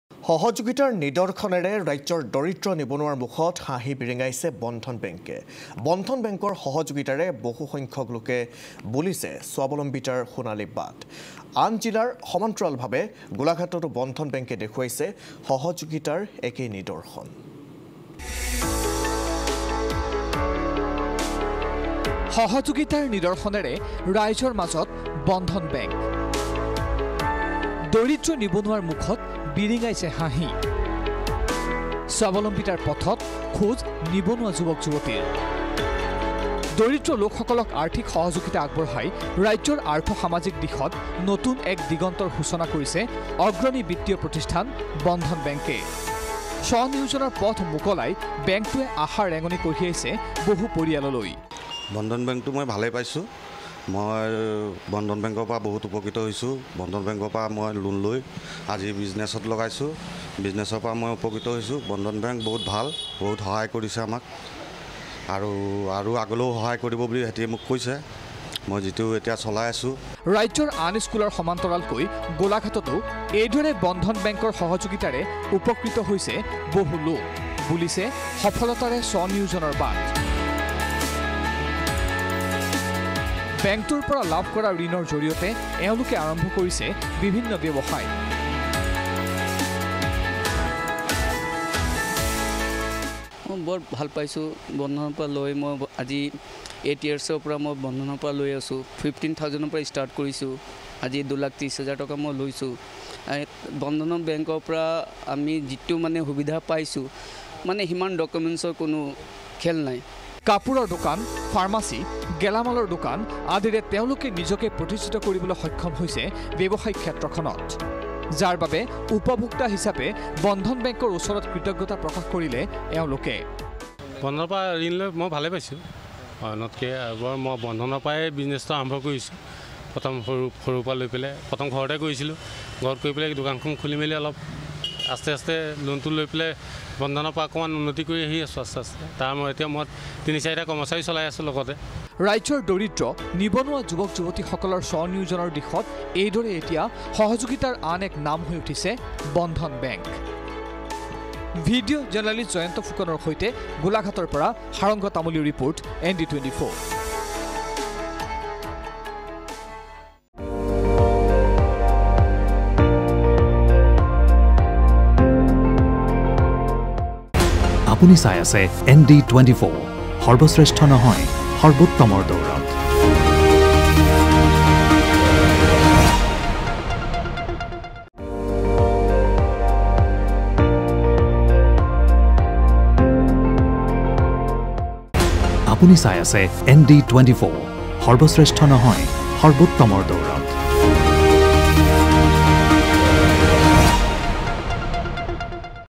সহযোগিতাৰ নিদৰ্শনেৰে ৰাইজৰ মাজত বন্ধন বেংক
সহযোগিতাৰ নিদৰ্শনেৰে ৰাইজৰ মাজত বন্ধন বেংক। দৰিদ্ৰ নিবনুৱাৰ মুখত বিৰিঙাইছে হাঁহি।